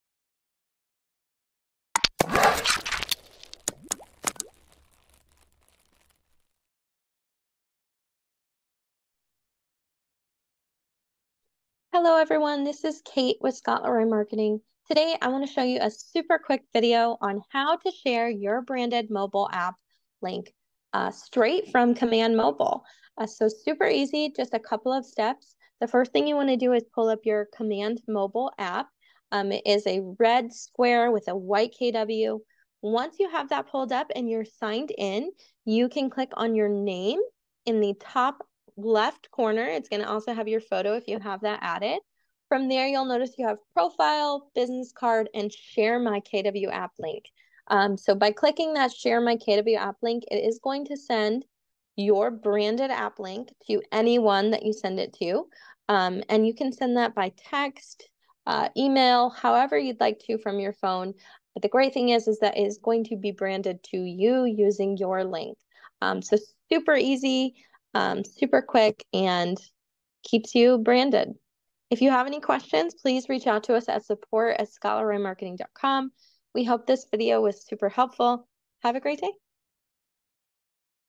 Tech Tip: Find and Share your KW App Link From Command Mobile
In this tip video we will show you how to find and share your branded Consumer app link from Command Mobile.